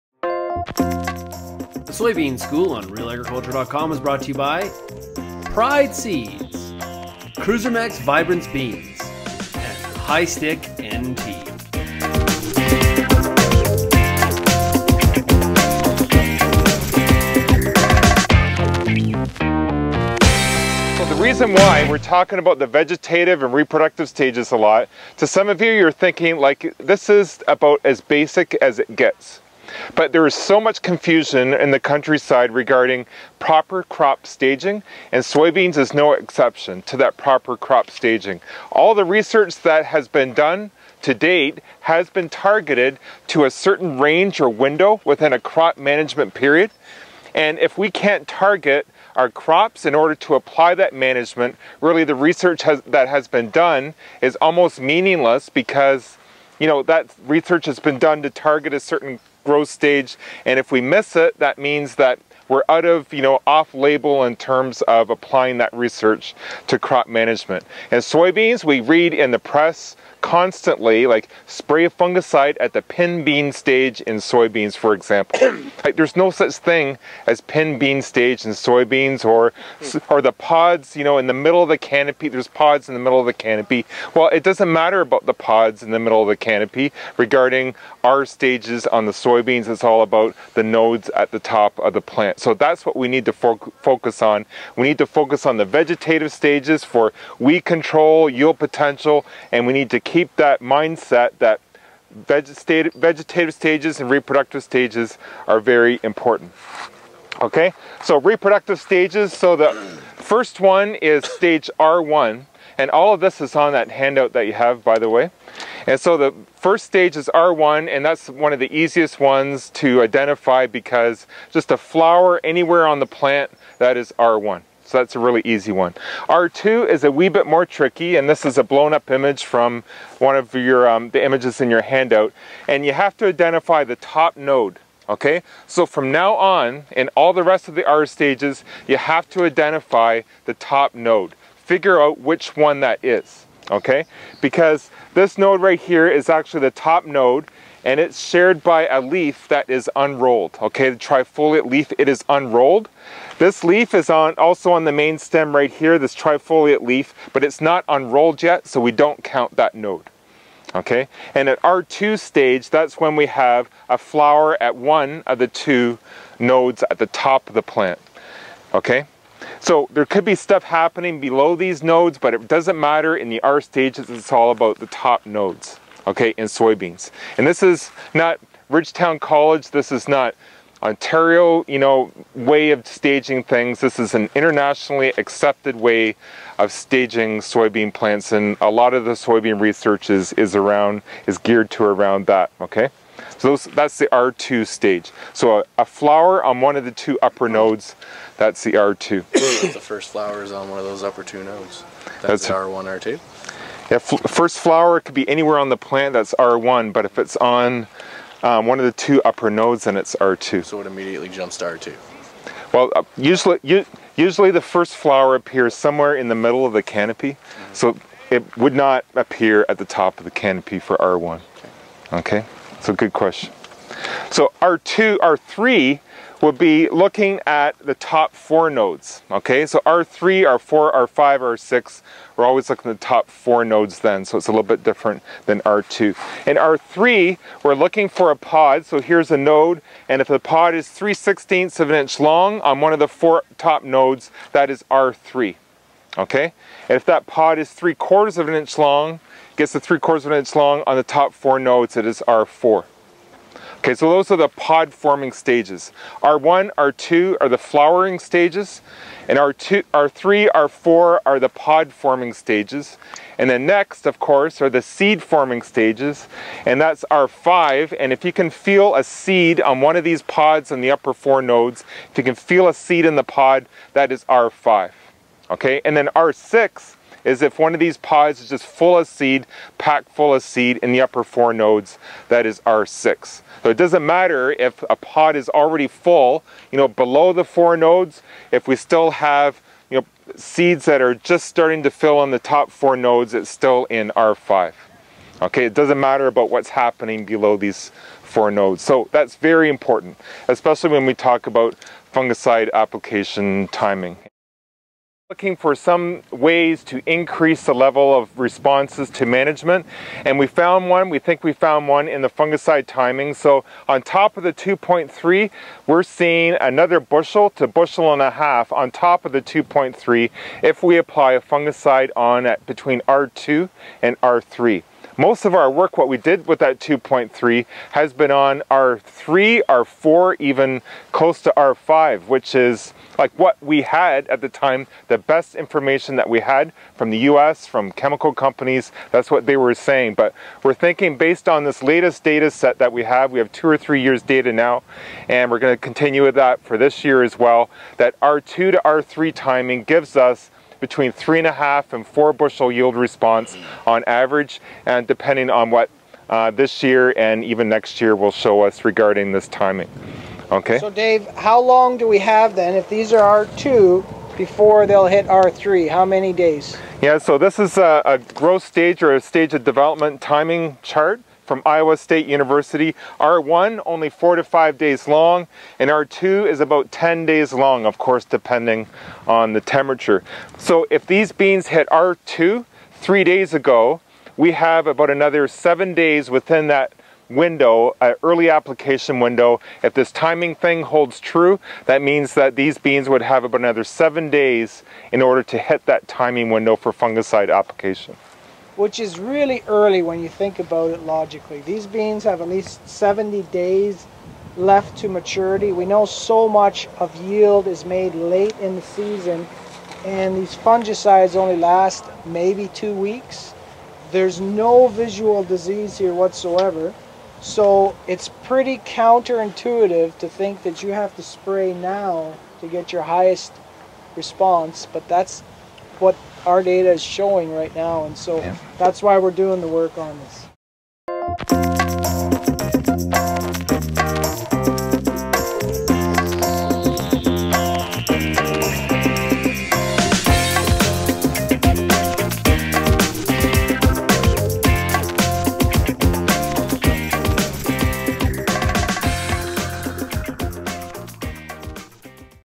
Soybean School - Clearing Up the Confusion on Proper Crop Staging
Dr David Hooker of the U of G Ridgetown and Horst Bohner of OMAFRA head out to the field to get a look at soybean staging.